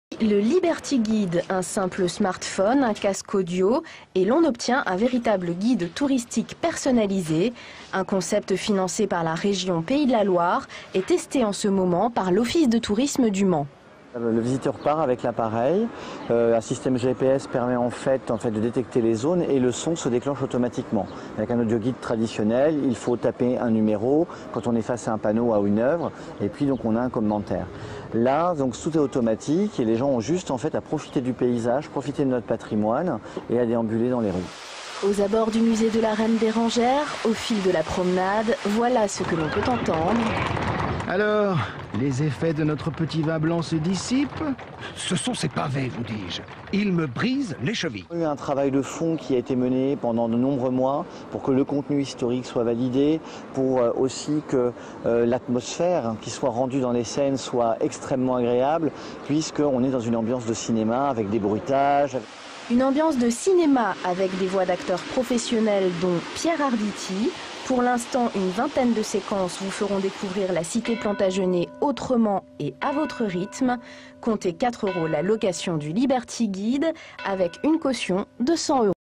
Découvrir le Mans grâce au Liberty Guide!
Sites webs/blogs, copiez cette vidéo avec le bouton "Intégrer" !
 Un guide audio qui se localise grâce au système GPS permet de découvrir autrement la ville du Mans